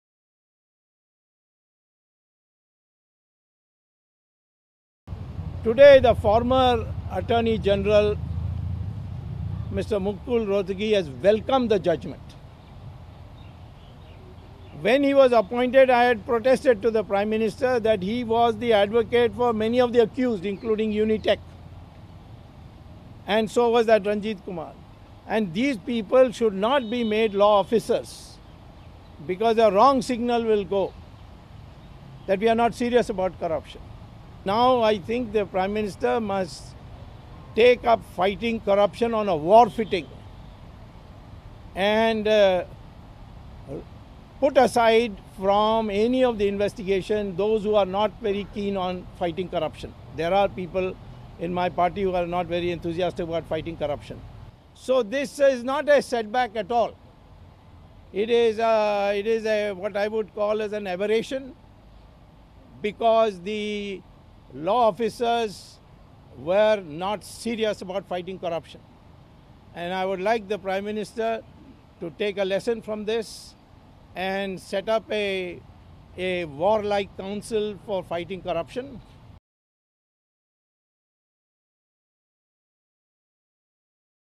2G Scam verdict: Subramanian Swamy says not a setback, law officers were not serious - Delhi News
New Delhi, Dec 21 (ANI): BJP leader Subramanian Swamy, who unearthed the 2G spectrum case, on Thursday, snubbed the verdict in the muti-crore scam, saying that it is not a setback, law officers were not serious.